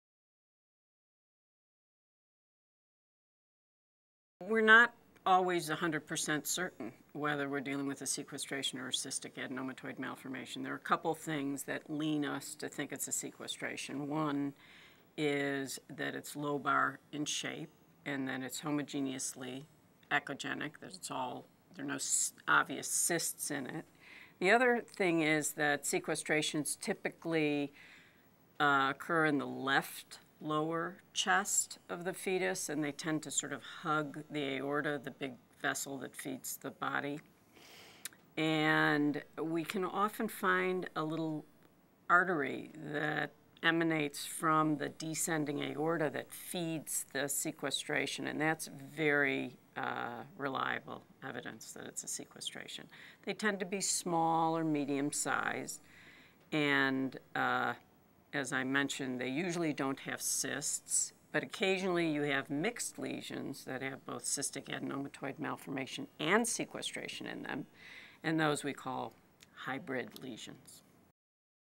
Pulmonary Sequestration - How is it different from CCAM?
The difference between Pulmonary Sequestration and Congenital Cystic Adenomatoid Malformation (CCAM). Presented by Ruth Goldstein, MD, Chief of Diagnostic Sonography for UCSF Radiology in cooperation with the UCSF Fetal Treatment Center